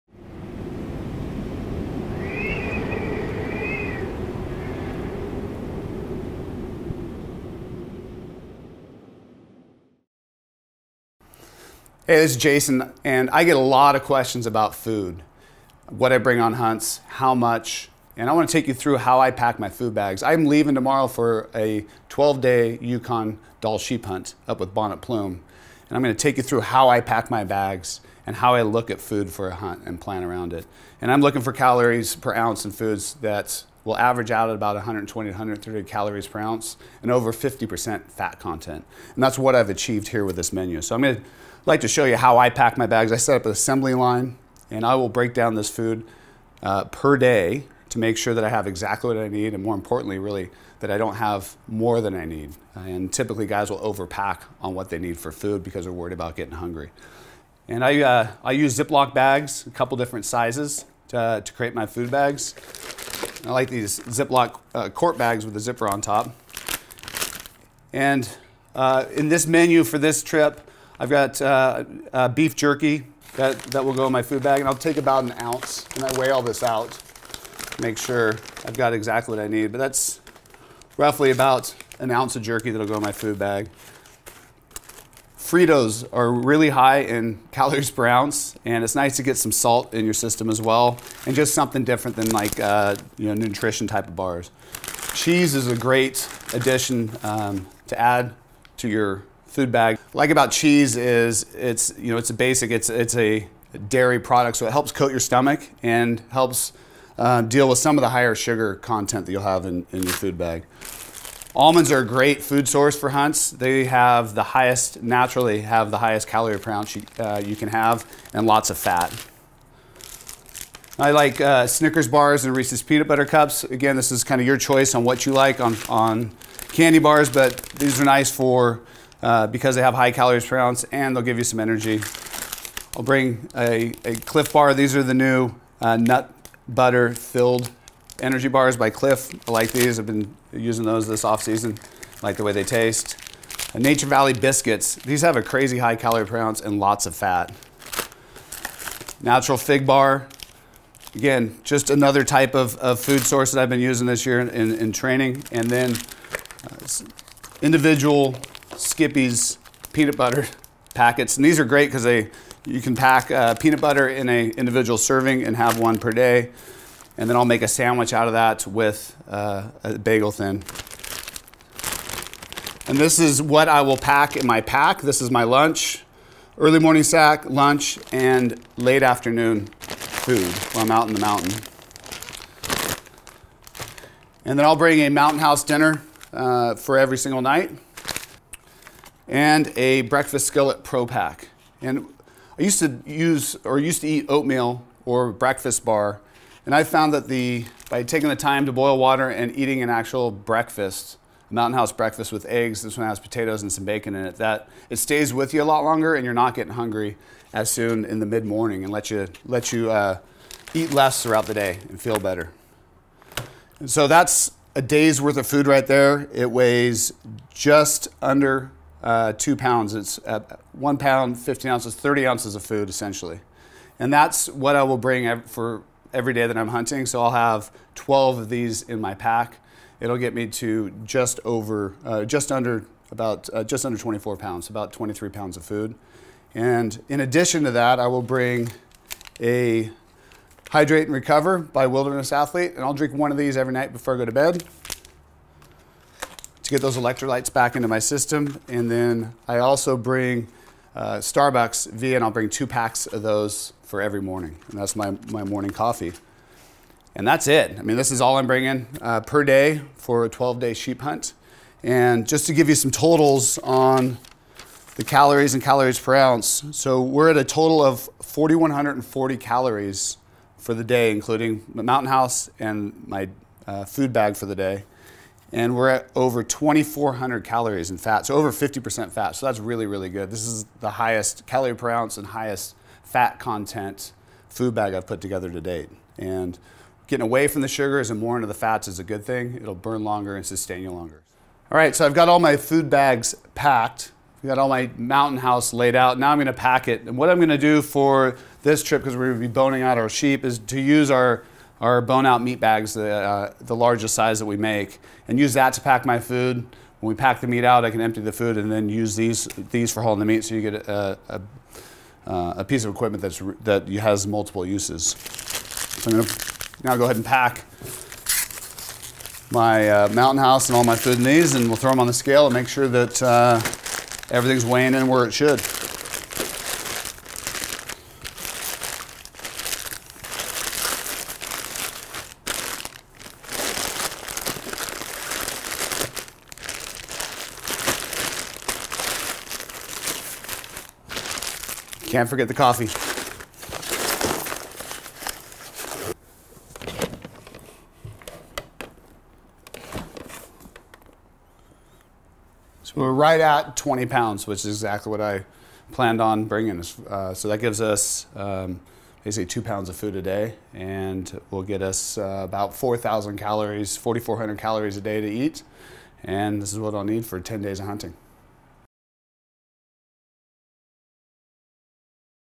Sheep Hunt Food Packing and Calorie Count
Jason reveals his process for choosing, packing, and weighing his food for a ten-day sheep hunt.  He goes into detail about specific foods, fat content, total calories and overall meal planning for long backpack hunts.

For his Yukon Sheep hunt, Jason packed 20 pounds of food providing him 4140 calories per day with 2400 calories from fat.

***Food List (Packed individually in one quart Ziploc bags)
Beef Jerky
Fritos
Individually wrapped Cheese
Lightly Salted Almonds
Snickers Bar
Reese’s Peant Butter Cups
Clif Bar Nut Butter Gilled
Nature Valley Biscuits
Fig Bars
Individual Serving Peanut Butter
Bagel Thins

Mountain House Dinners
Mountain House Pro Pak Breakfast Skillets

Wilderness Athlete Hydrate and Recover
Starbucks Via Instant Coffee